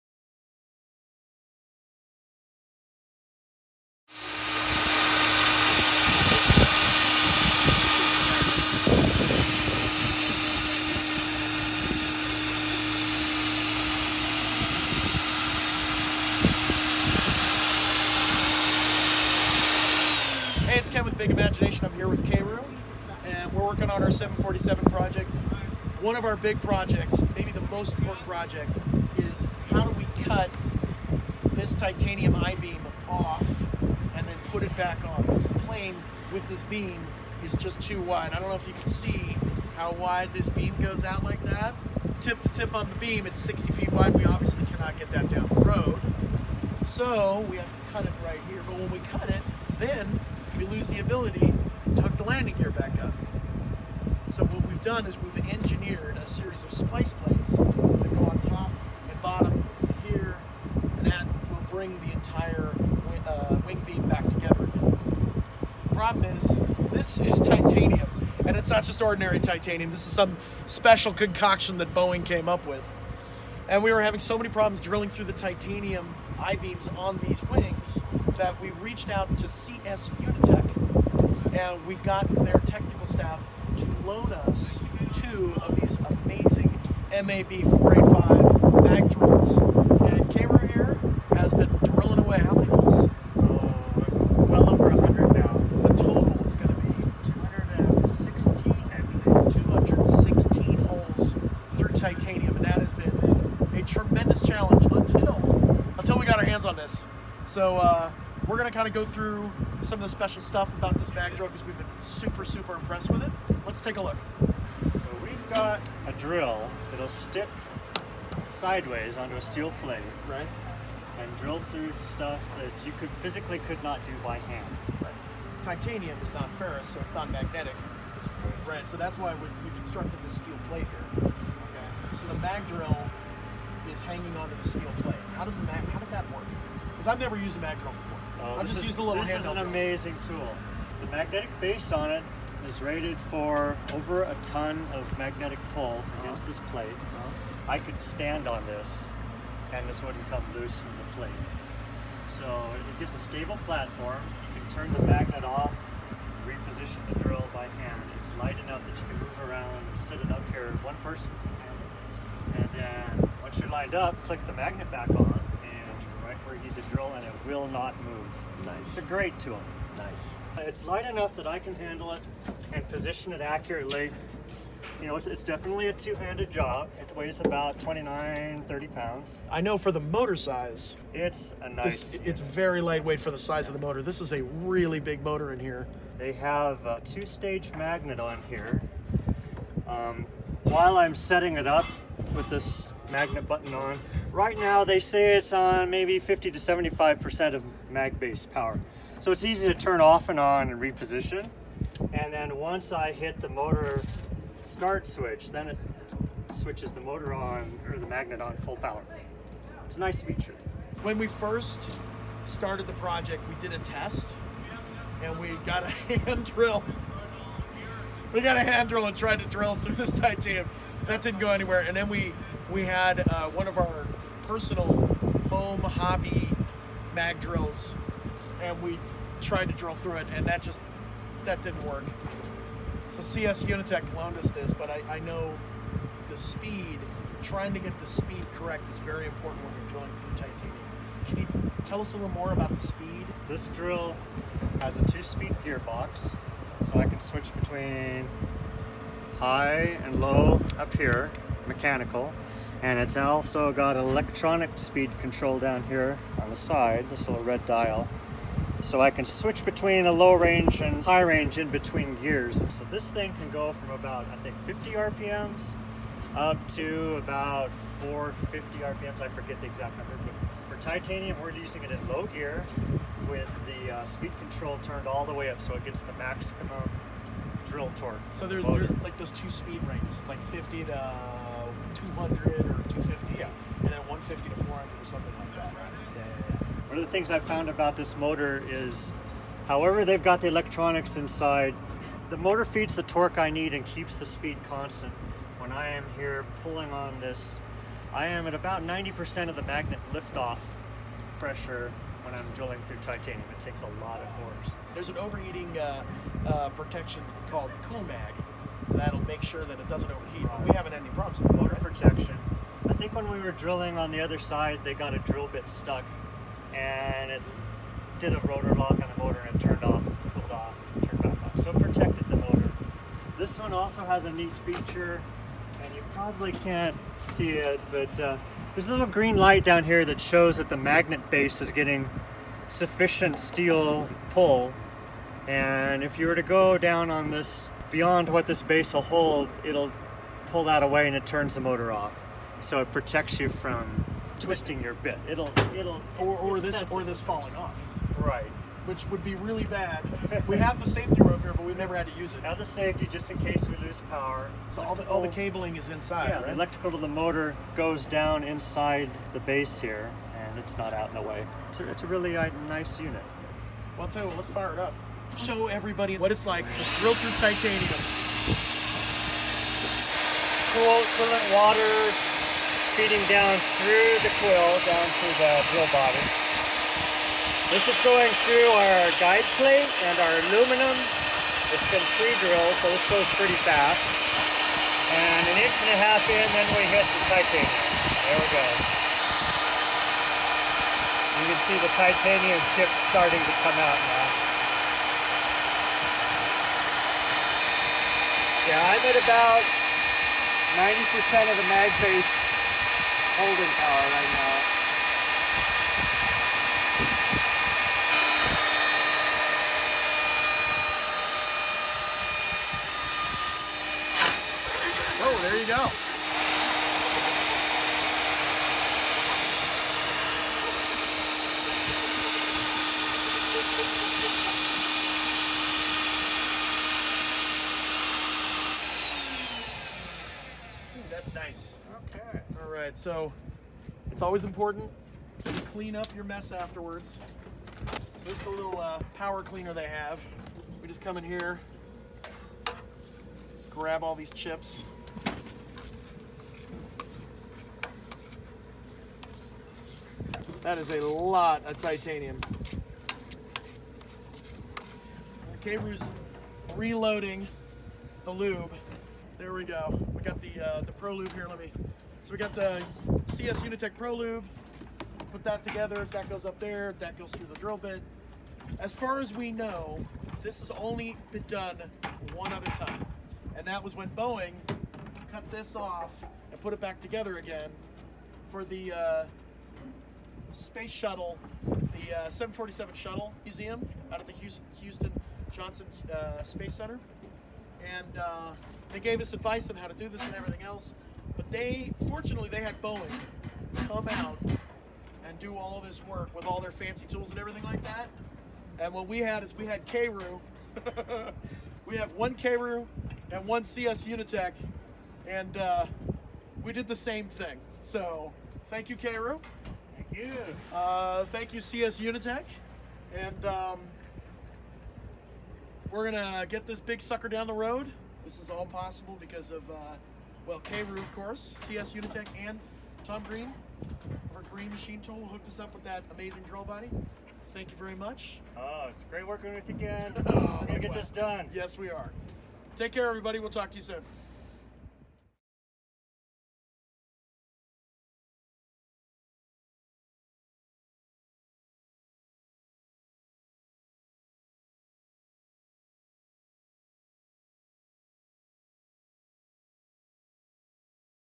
Drilling a Boeing 747 with a CS Unitec Mag Drill - Big Imagination MAB 485 Testimonial
When Big Imagination needed to drill though titanium I-beams for their Boeing 747 Project, they turned to CS Unitec for a tool that was up to the task. The MAB 485 mag drill delivered, and in this video they show you how.